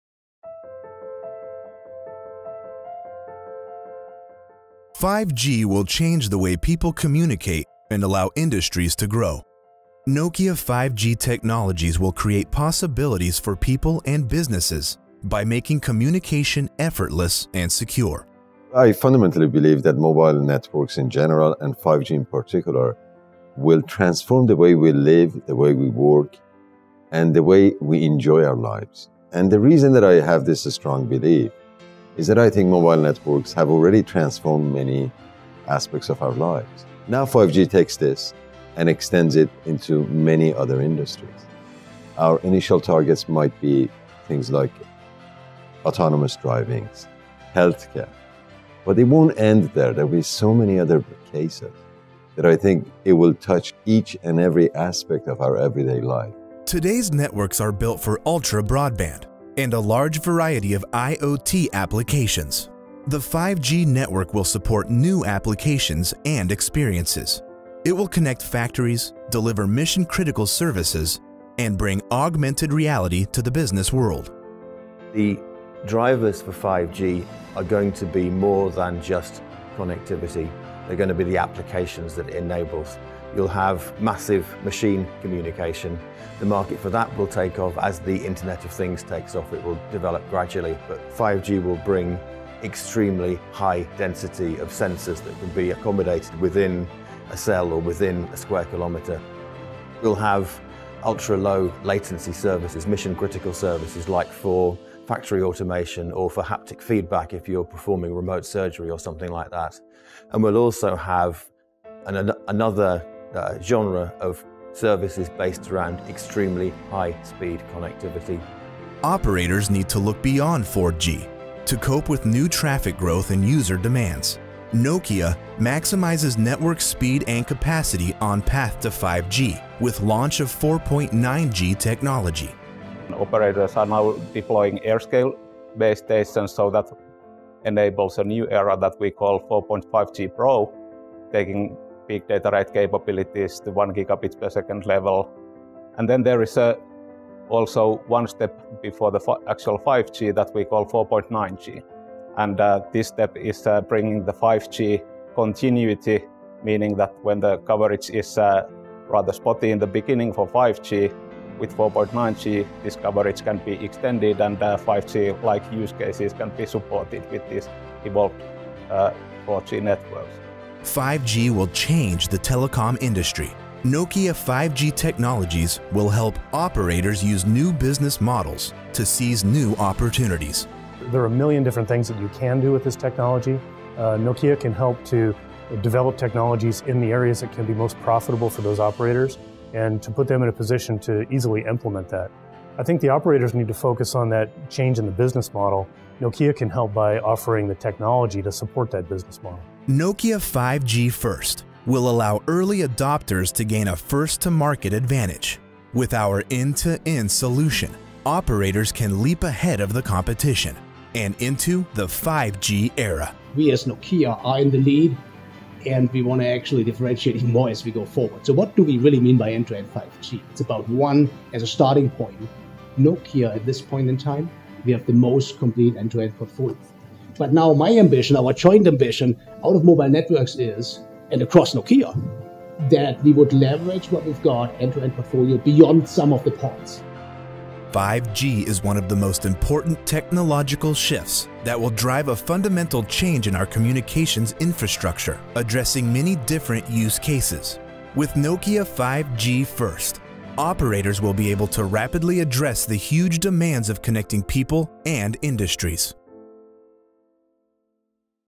Nokia 5G FIRST. Making 5G a commercial reality.
5G will change the way people communicate and enable growth in many industries, ranging from the automotive, entertainment, healthcare, and manufacturing industries.  The biggest difference between 5G and today’s systems is the diversity of use-cases and experience that 5G networks will support.  More than just providing mobile broadband, 5G will connect factories, deliver mission-critical services, and bring virtual augmented reality to the consumers and business world.  In this video Nokia executives and industry leaders discuss how we are transform human experiences with effortless, simple and dependable technology and how together we can shape the future of human possibilities.